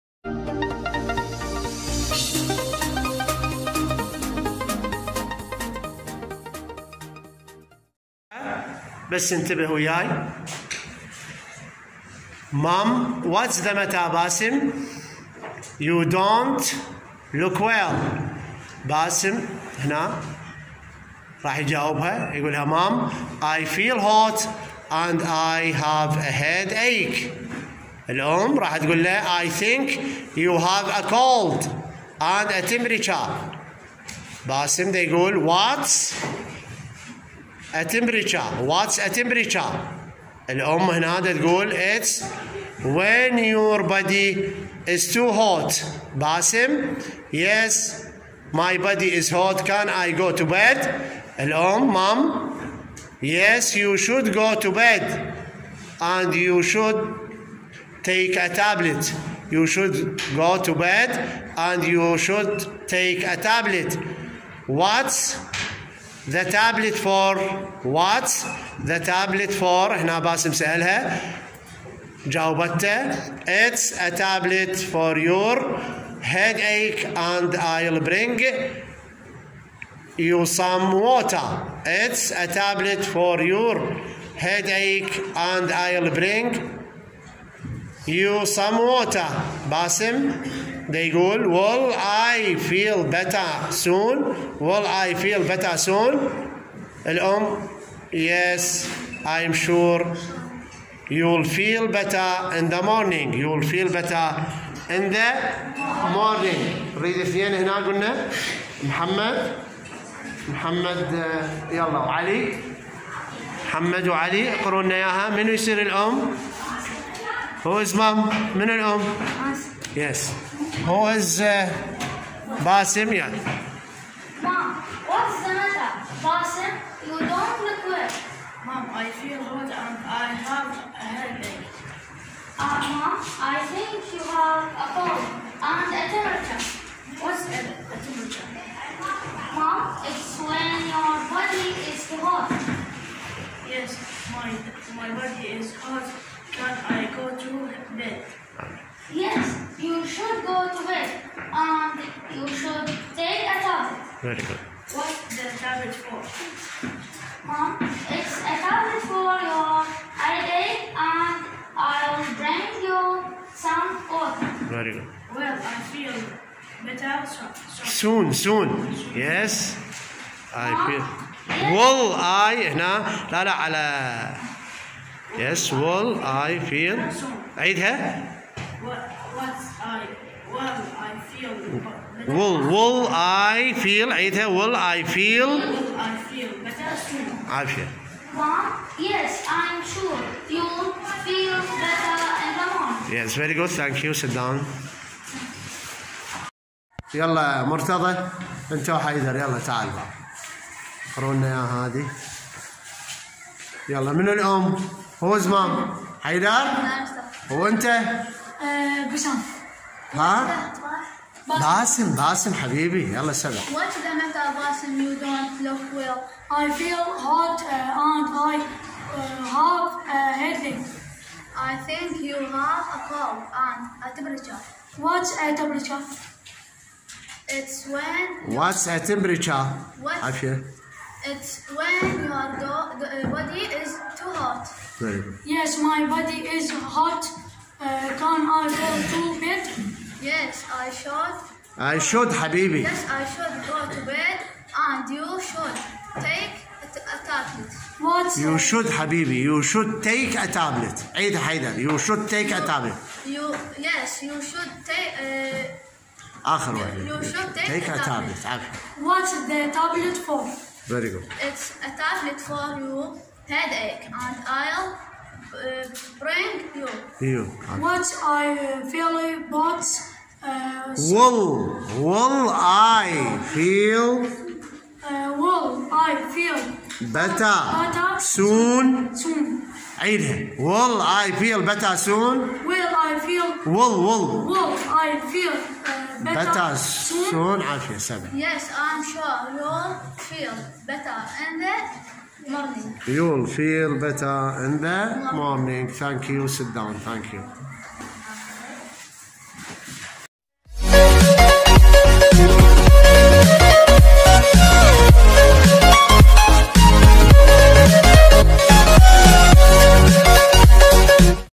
جانب من احد دروس اللغه الانكليزية الصف السادس الوحدة3 في كتاب التلميذالدرس2 ص41 مدرسة الحسين(ع)...
مدرسة الحسين (ع) الصف السادس الابتدائي في يوم الثلاثاء المصادف 12/14 /2021 أثناء الدوام الصباحي.